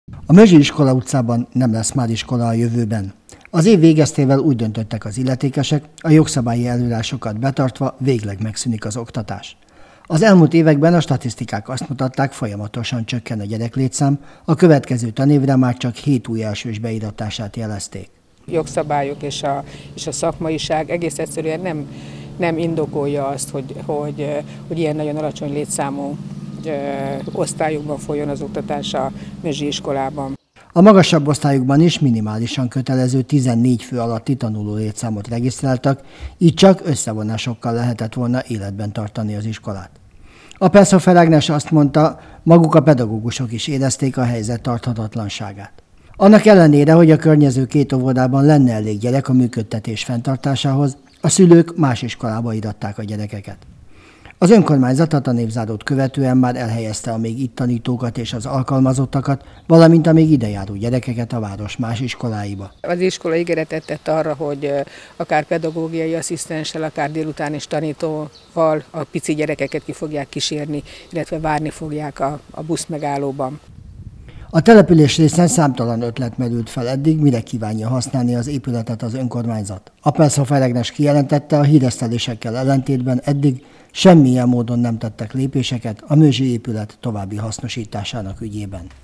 Iskolabezárás Mözsön- nem volt elég gyerek a folytatáshoz
A tanév végével végleg bezárt a Tolna-Mözsi Általános Iskola. Az évek folyamán egyre csökkenő gyereklétszám miatt már nem lehetett életben tartani a városrész oktatási intézményét. A hírekkel ellentétben az épülettulajdonos Önkormányzatban egyelőre nem beszéltek az épület további hasznosításáról.